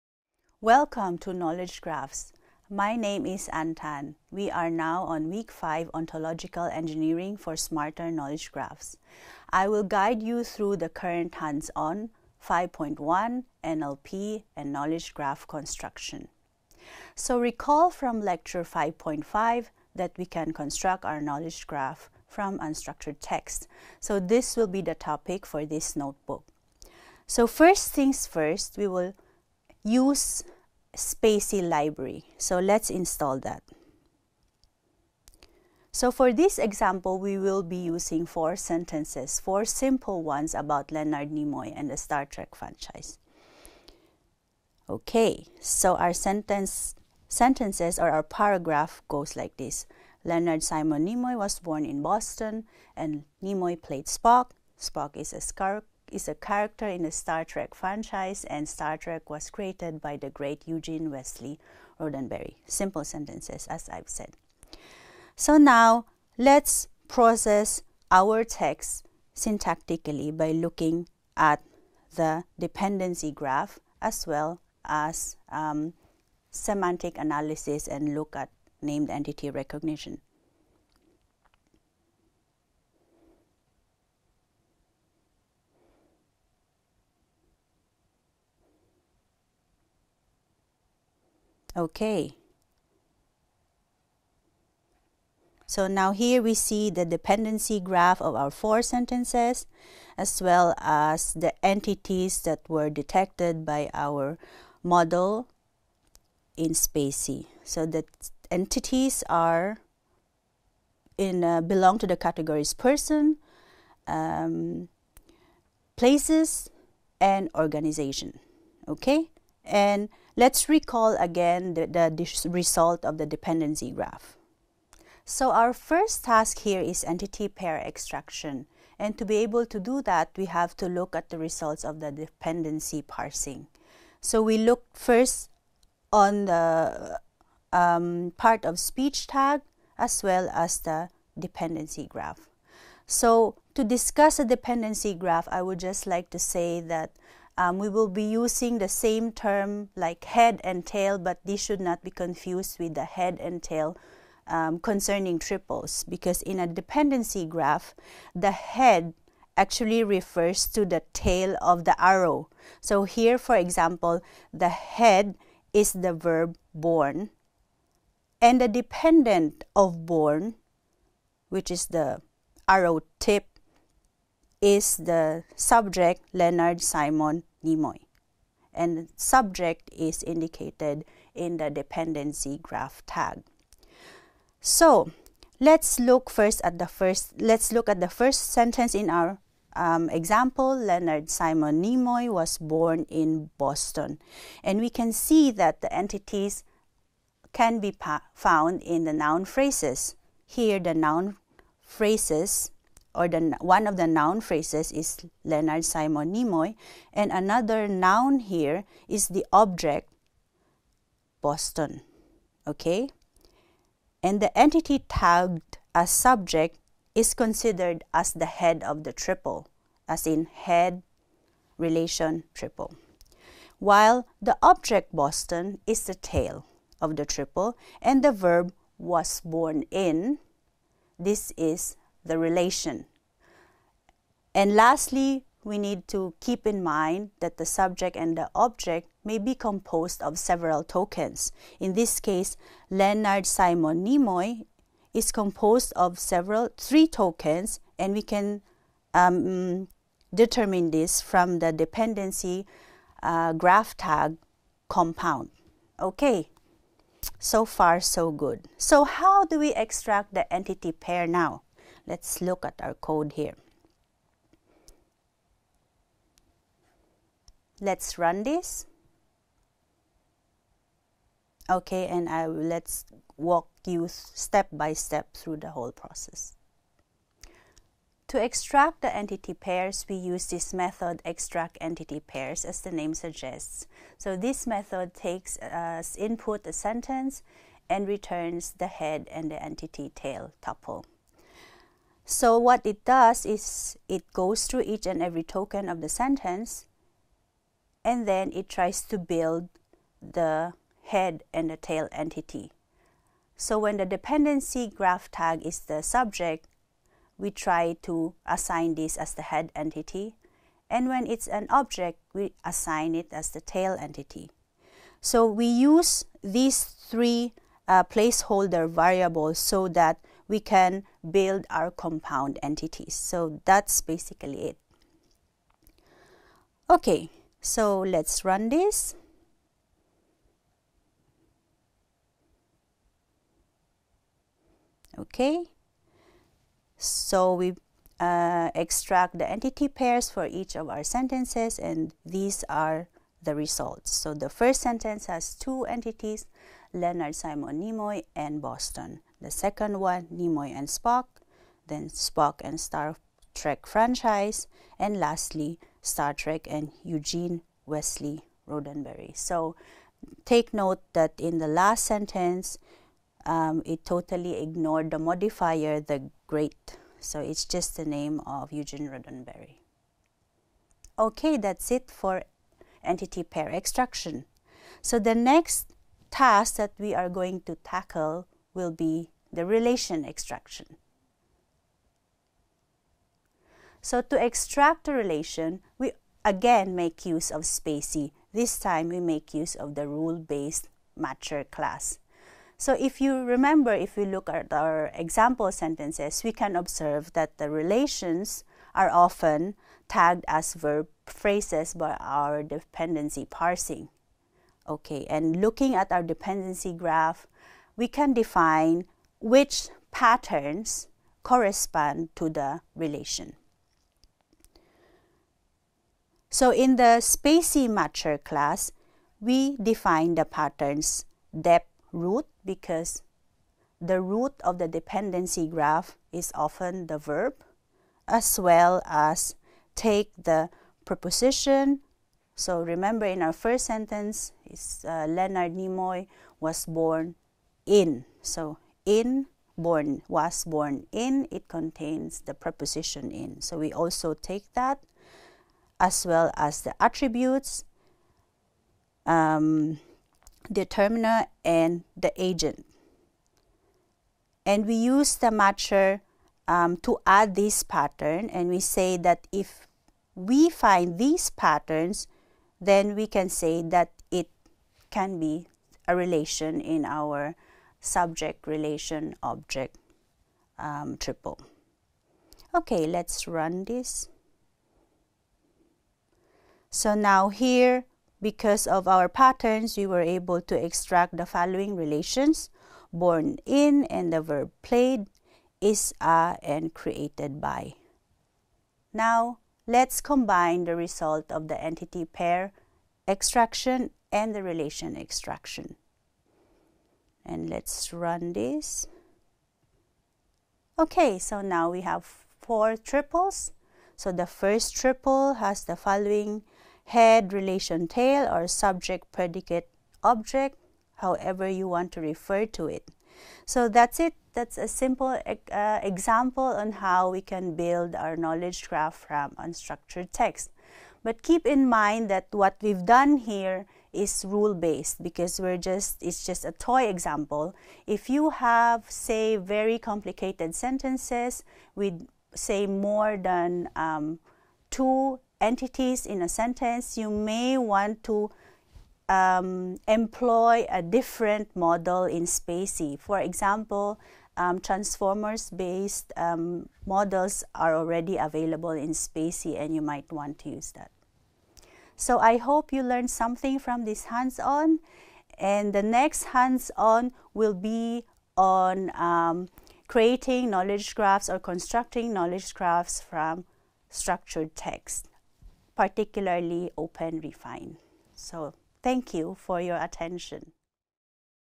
Knowledge Graphs - Hands-On 5.1 NLP and Knowledge Graph Construction
Knowledge Graphs - Foundations and Applications
Ontological Engineering for Smarter Knowledge Graphs
Speakers: Mary Ann Tan